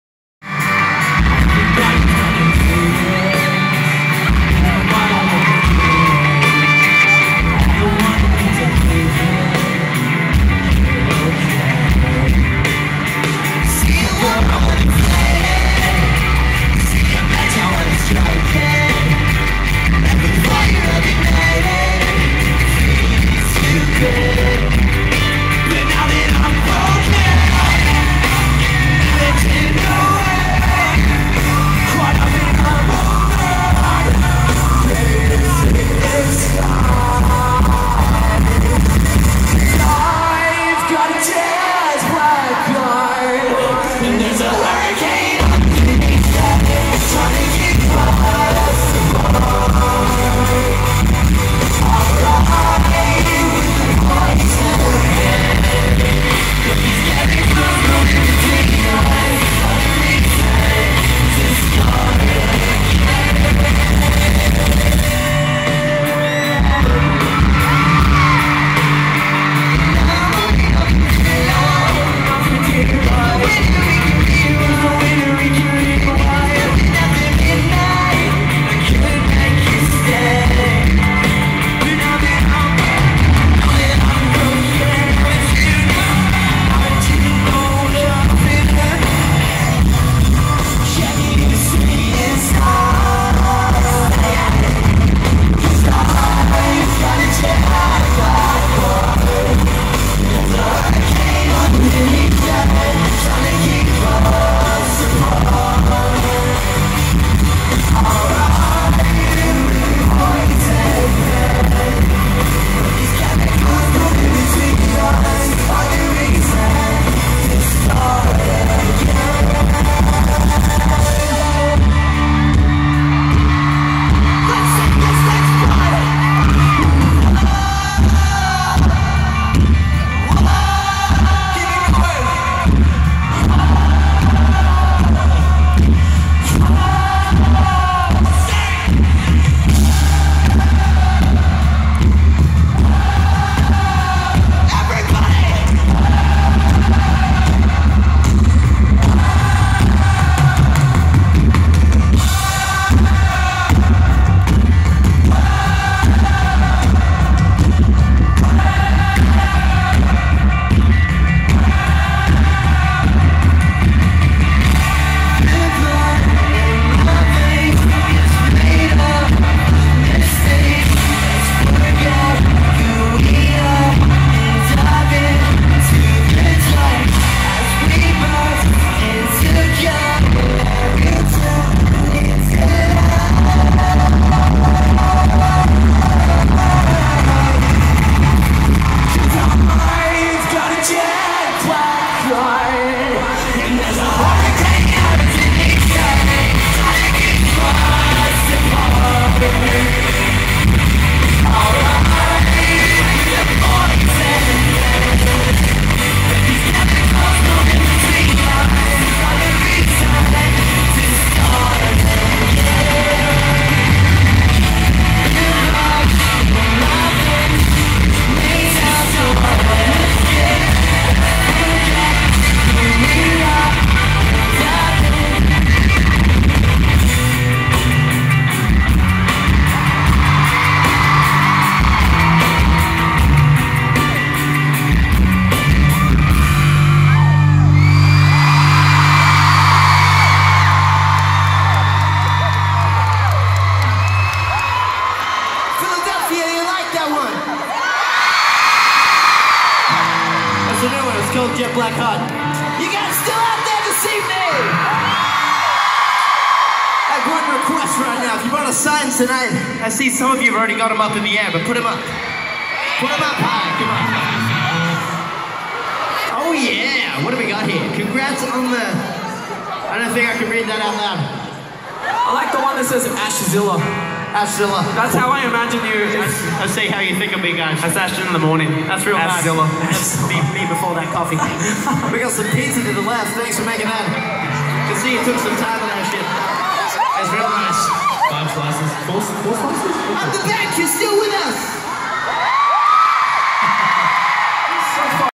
5SOS @ SUSQUEHANNA BANK CENTER
Took my daughter to see here favorite band.